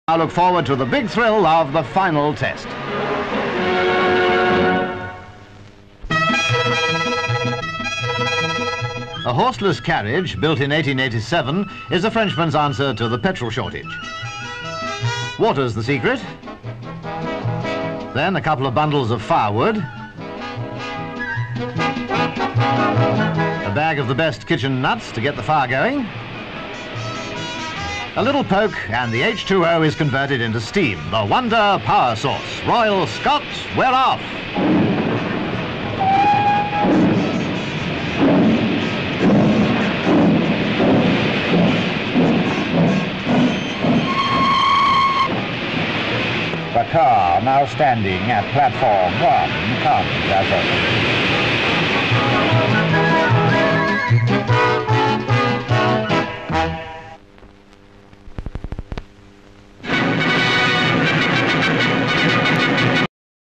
FULL STEAM AHEAD
(25 Feb 1957) A horseless carriage built in 1887 is a Frenchman's answer to the petrol shortage.